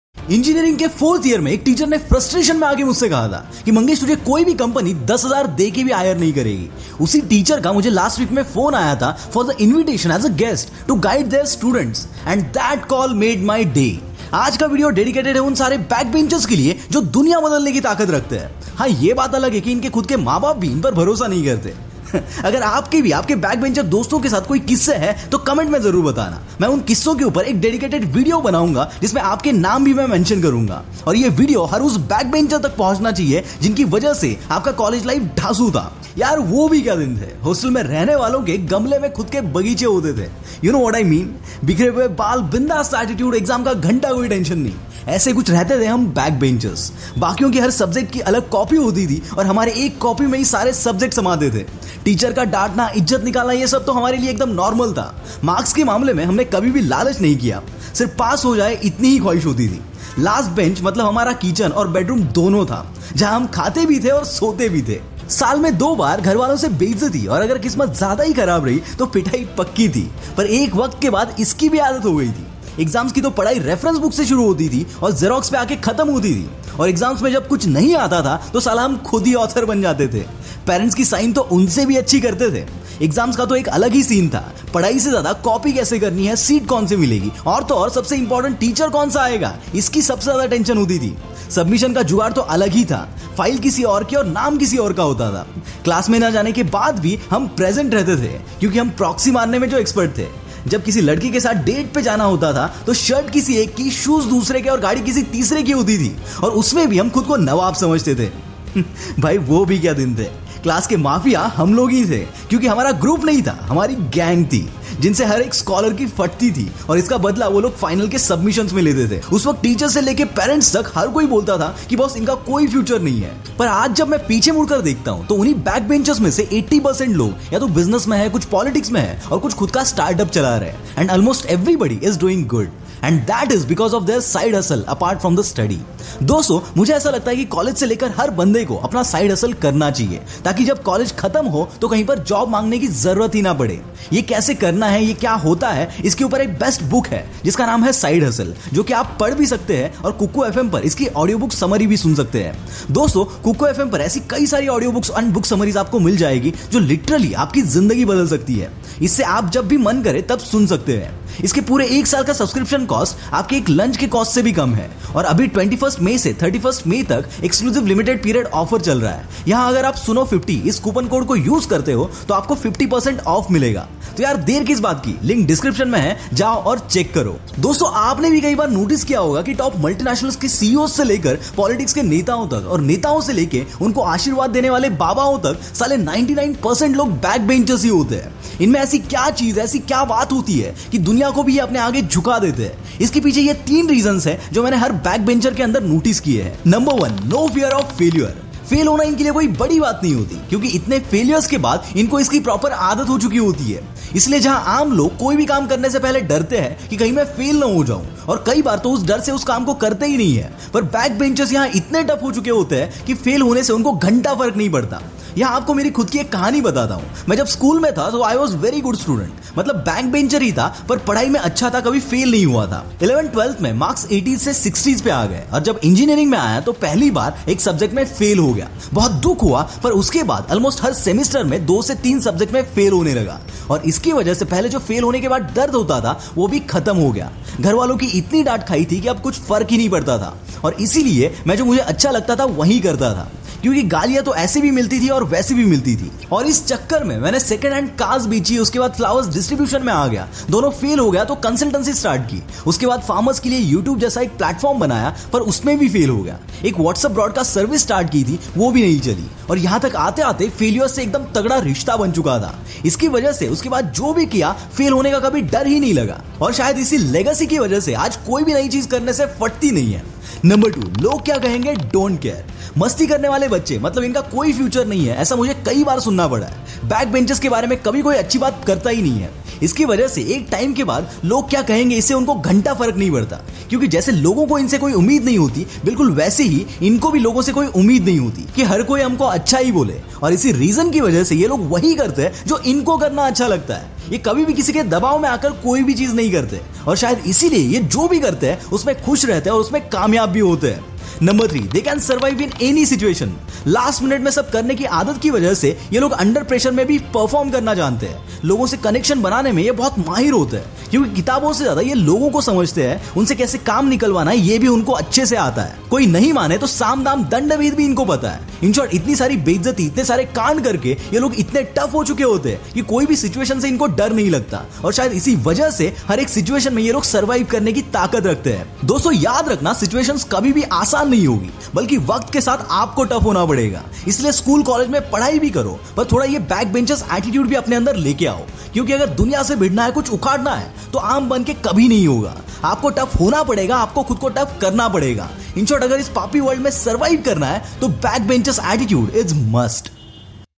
इन 3 Reasons की वजह से Backbenchers कामयाब होते है | Backbenchers by the willpower star
Backbenchers don’t have any future, most of the time we heard it from multiple people but this is not at all true. Because I personally believe if you want to survive through any difficulties any tough period you should have that backbenchers attitude in yourself.

This video is very close to my heart because when was writing the script of a video I experienced, I remembered all of my college life and that brought a smile to my face I am sharing this video with all of my college friends.